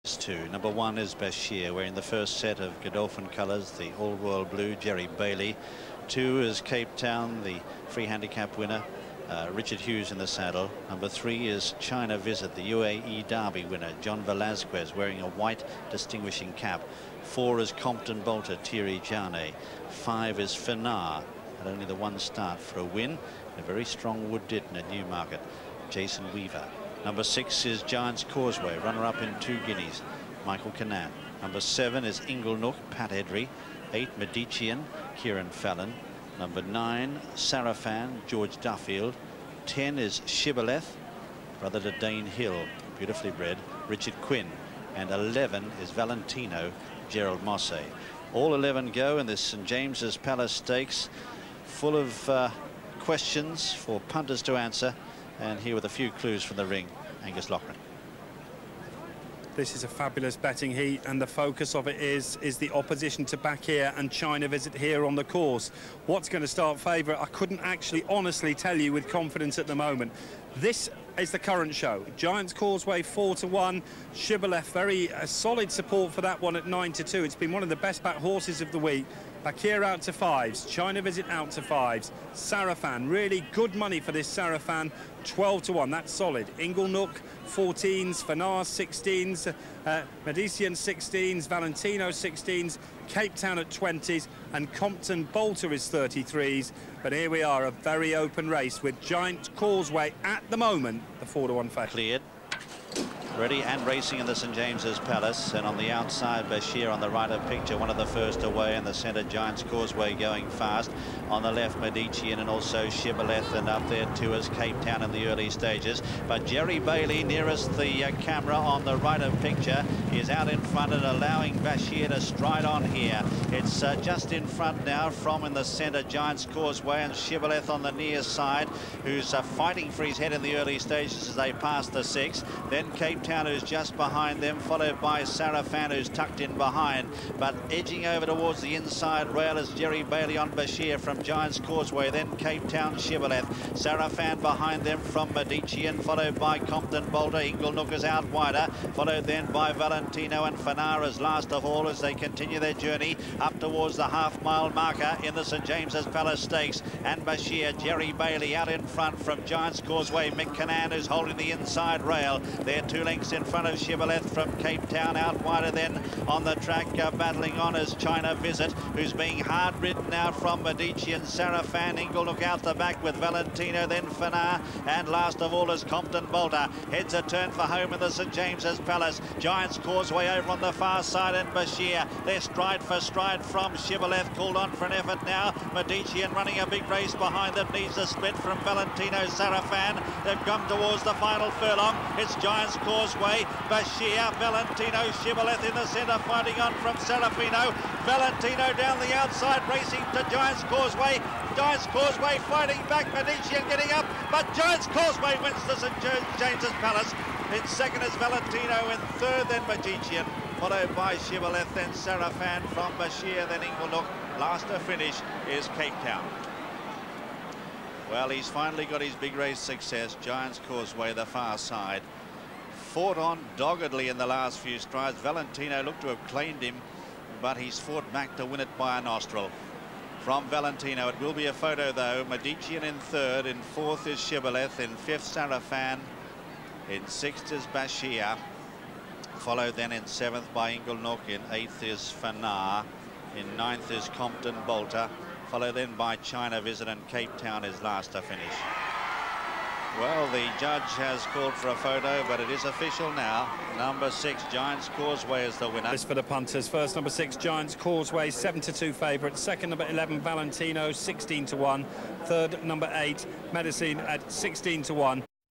2000 St James's Palace Stakes
Date: Tuesday 20th June 2000 |
Course: Ascot |
Distance: 1m |
Going: Good to Firm |
Winning Time: 1m 42.61s |
Winner: Giant's Causeway (Mick Kinane) [A P O'Brien] |
Runners: 11